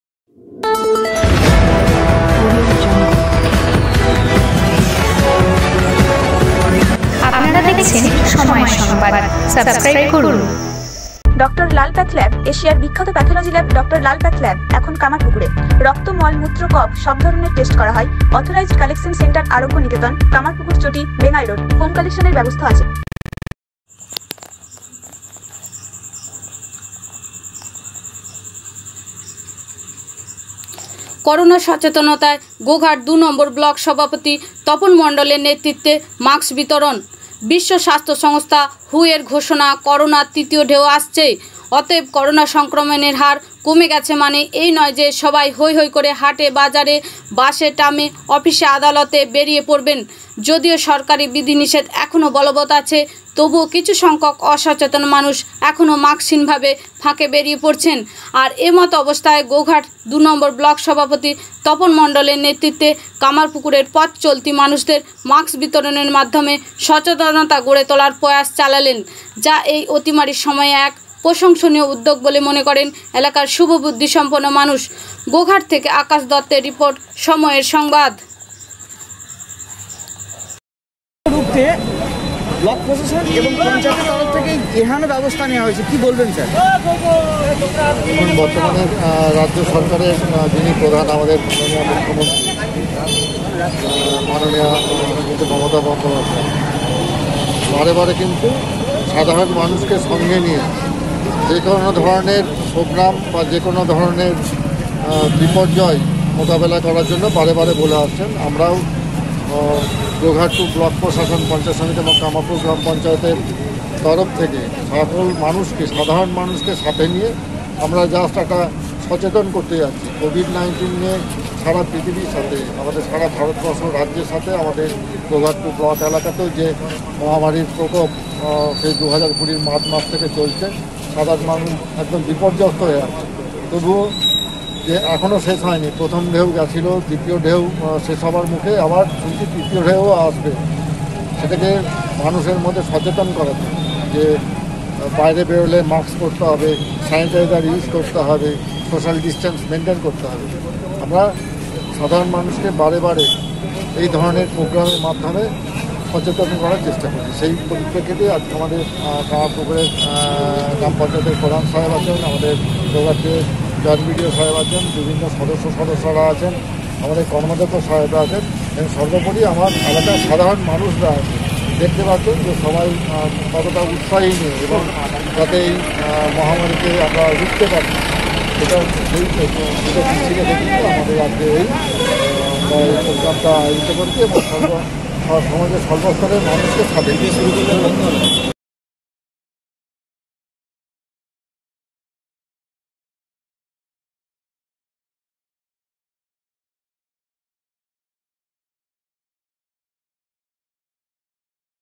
করোনা সচেতনতায় গোঘাট দু'নম্বর ব্লক সভাপতি তপন মন্ডল- এর নেতৃত্বে মাক্স বিতরণ।
বিশ্ব স্বাস্থ্যসংস্থা হু- এর ঘোষণা করোনা তৃতীয় ঢেউ আসছেই , অতএব করোনা সংক্রমনের হার কমে গেছে মানেই এই নয় যে সবাই হই হই করে হাটে-বাজারে বাসে ট্রামে অফিস-আদালতে মানুষ হই হই করে বেরিয়ে পড়বেন, যদিও সরকারি বিধি নিষেধ এখনো বলবত আছে তবুও কিছু সংখ্যক অসচেতন মানুষ এখনো মাক্স হীন ফাঁকে বেরিয়ে পড়ছেন আর এমতাবস্তায় গোঘাট 2 নম্বর ব্লক সভাপতি তপন মন্ডল এর নেতৃত্বে কামারপুকুরে পথ চলিতে মানুষদের মাক্স বিতরণের মাধ্যমে সচেতনতা গড়ে তোলার প্রয়াস চালালেন যা এই অতিমারির সময়ে এক প্রশংসনীয় উদ্যোগ বলেই মনে করেন এলাকার শুভবুদ্ধি সম্পন্ন মানুষ।

গোঘাট থেকে আকাশ দত্তের রিপোর্ট // সময়ের সংবাদ //